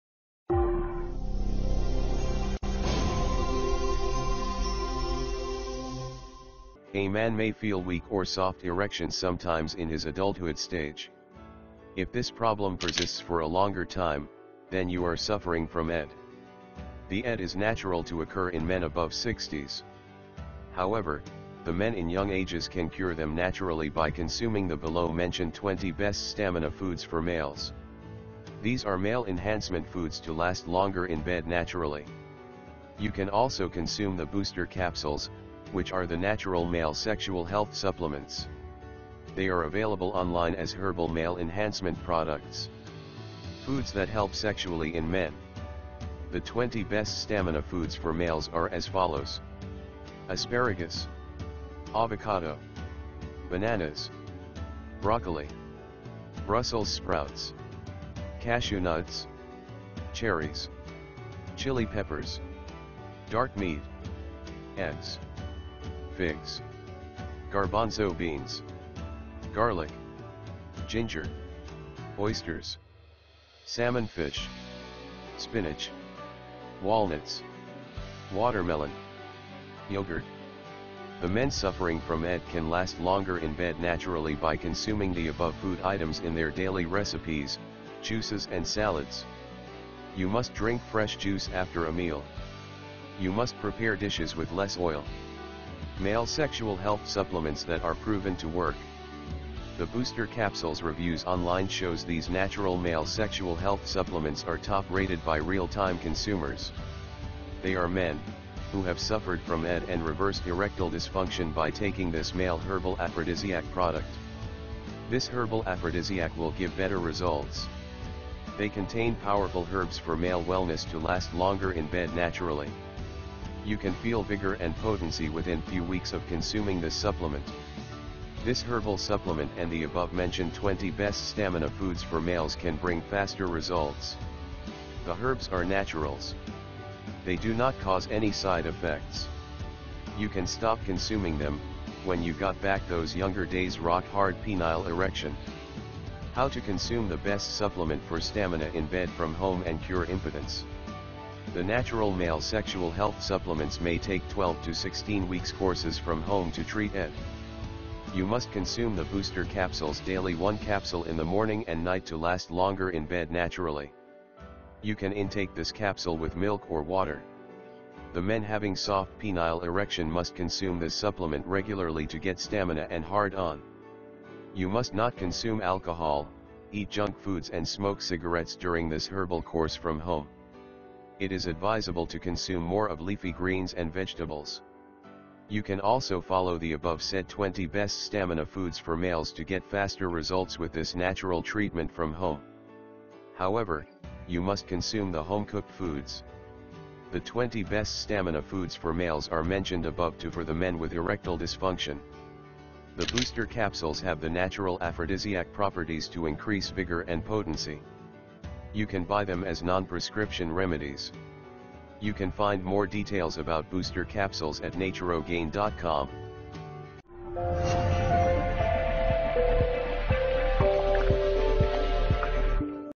Best Natural Male Stamina Booster Pills, Foods to Last Longer in Bed
Find information about the best natural male stamina booster pills and foods to last longer in bed

In this video, we have discussed the best natural male stamina booster pills and foods to last longer in bed.

Booster capsules are purely natural supplements helpful in improving male stamina and power in men to last longer in bed.